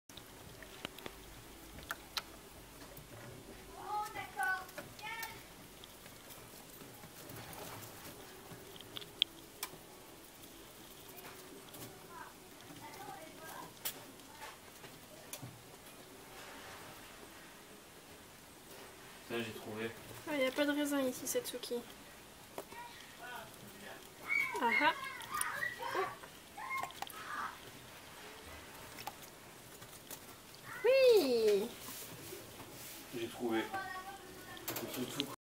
Satsuki et la piscine aux raisins
Yubabâ trempe ses fesses dans l'eau, puis Satsuki qui nous fait une démonstration de pêche aux raisins secs!